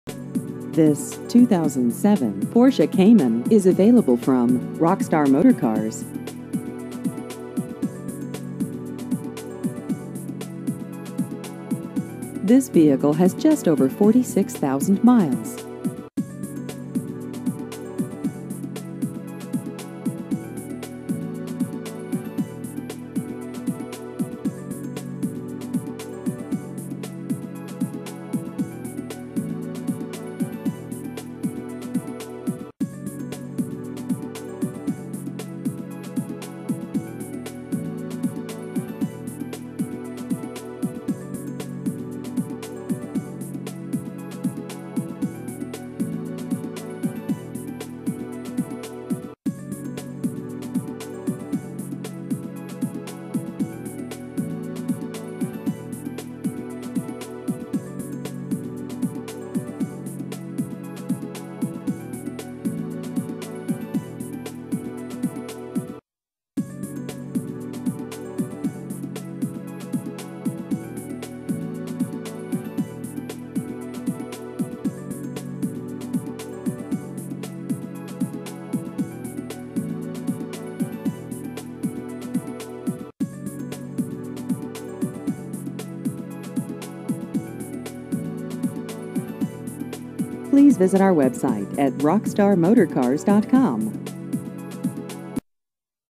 2007 Porsche Cayman Used Cars Nashville TN
This 2007 Porsche Cayman is available from Rockstar Motorcars.